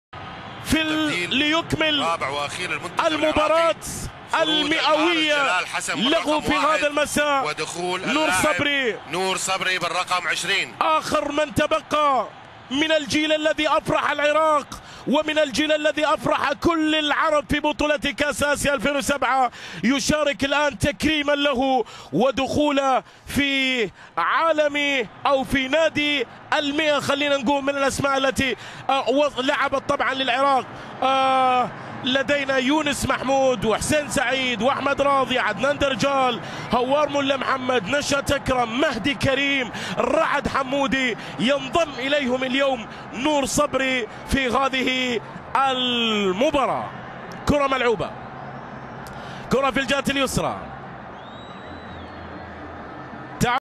لحظة دخول نور صبري الحارس الاسطورة
لحظة دخول نور صبري الحارس الاسطورة ملعب البصرة في مباراة سوريا بطولة الصداقة الدولية في مباراته المؤوية لحظة تشجيع الجماهير .. ما رأيكم ؟